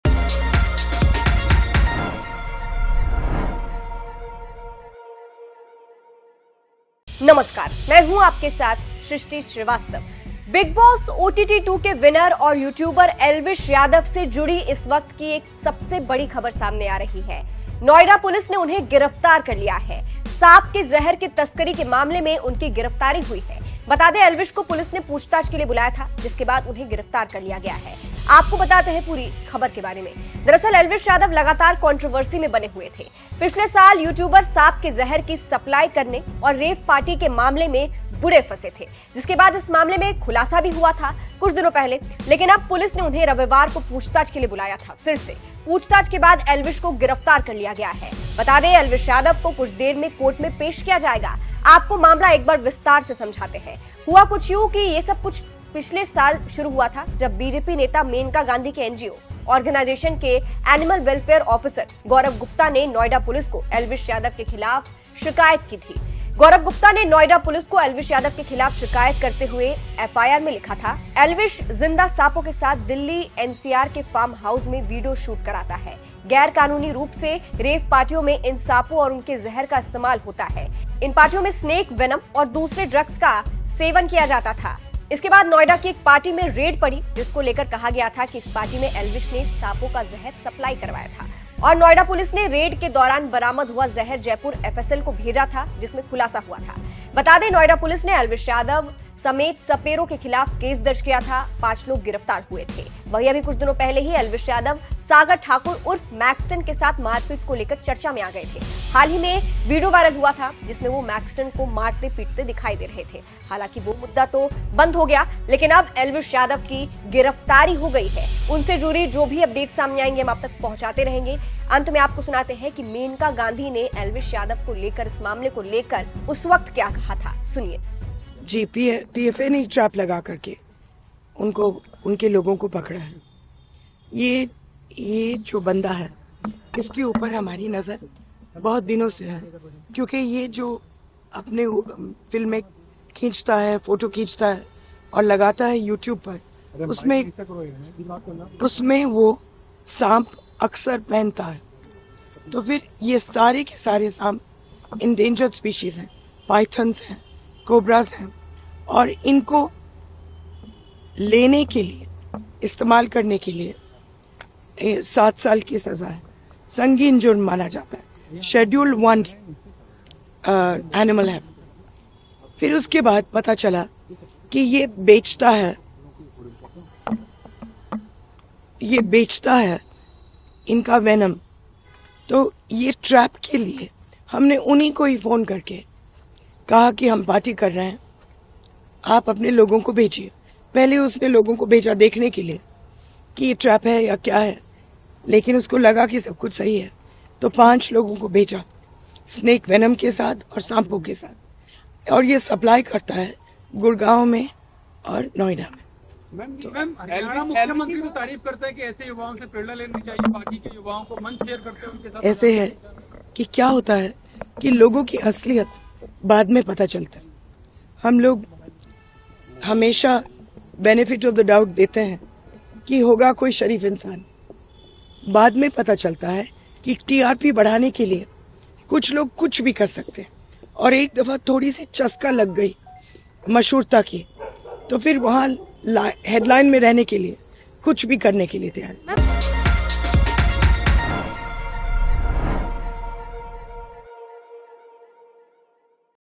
आखिरकार नोएडा पुलिस ने Elvish Yadav को कर ही लिया गिरफ्तार
बिग बॉस ओटीटी 2' के विनर और यूट्यूबर एल्विश यादव से जुड़ी इस वक़्त एक बड़ी खबर सामने आ रही है। नोएडा पुलिस ने उन्हें गिरफ्तार कर लिया गया है। सांप के जहर की तस्करी के मामले में उनकी गिरफ़्तारी हुई है। बता दे, एलवीश को पुलिस ने पूछताछ के लिए बुलाया था जिसके बाद उन्हें गिरफ्तार कर लिया गया है।

Jogi
Shrishti